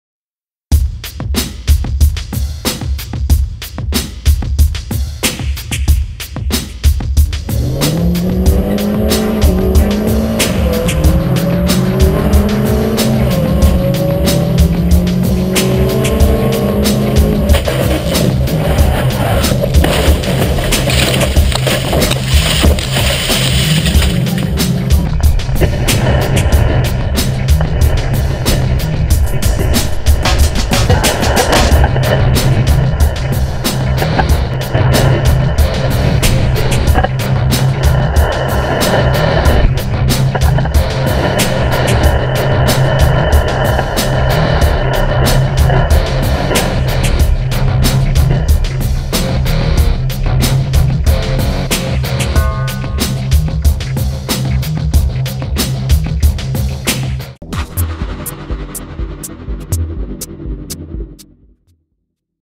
Mini Rollover Crash - GoPro HD 60FP
Thanks to Hayfisher Productions for filming this incredible crash. Check Them Out! Filmed from a mini GoPro HD on board camera! Filmed in R1 (840 x480) 60FPS. The slomo footage has been slowed down to 20% of the original speed. Luckily the driver walked away without major injuries! Always use safety gear that conforms to race rules! GoPro HD AVAILABLE HERE!!